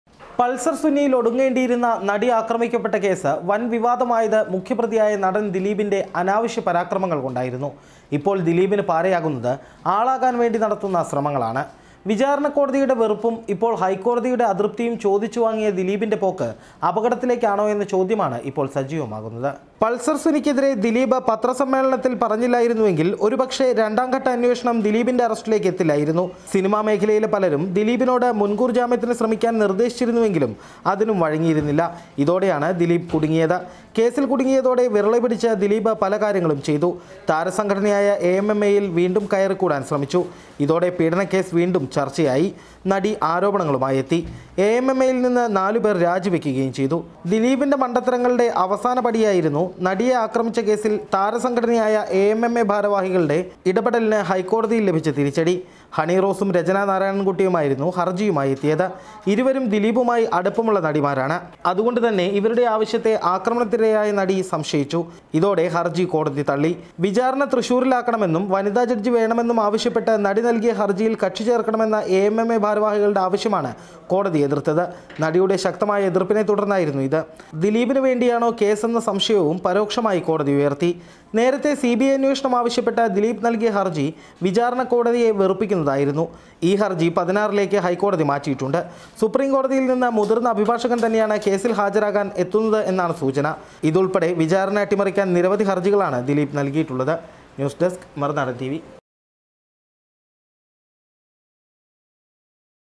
നടി ആക്രമിക്കപ്പെട്ട സംഭവത്തിനുശേഷം ദിലീപ് I Dileep
പള്‍സര്‍ സുനിയില്‍ ഒടുങ്ങേണ്ടിയിരുന്ന നടി ആക്രമിക്കപ്പെട്ട കേസ് വന്‍ വിവാദമായത് മുഖ്യപ്രതിയായ നടന്‍ ദിലീപിന്റെ അനാവശ്യ പരാക്രമങ്ങള്‍ കൊണ്ടായിരുന്നു